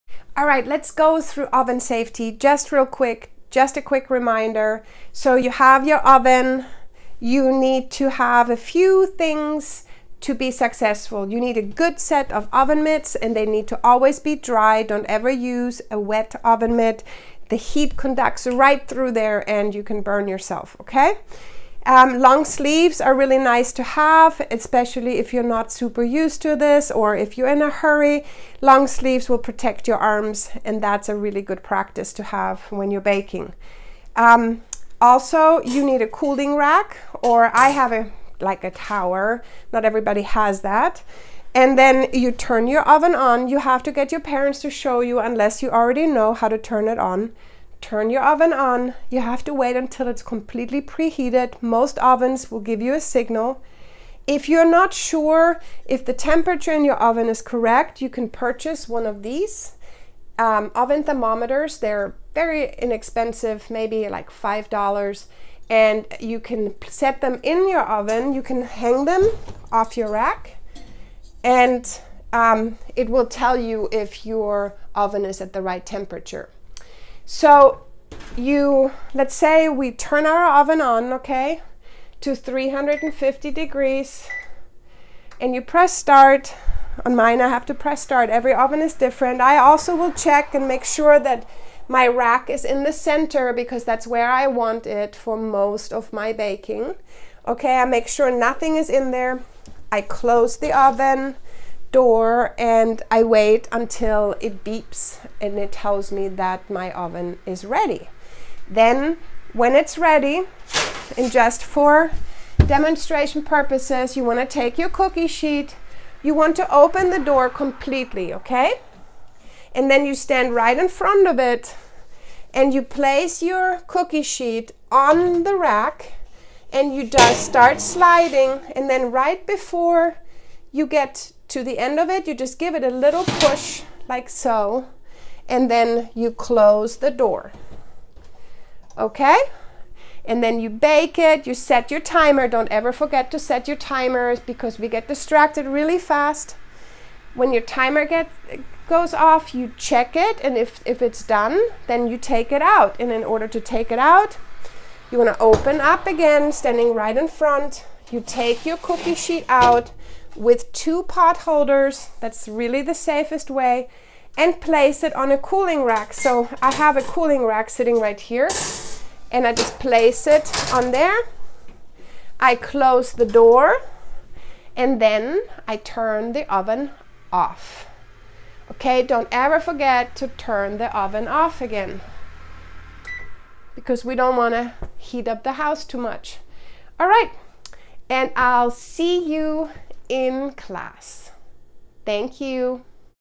Oven Safety Kids Culinary School by Bake Austin
In this video we will go over the basics on oven safety for kids.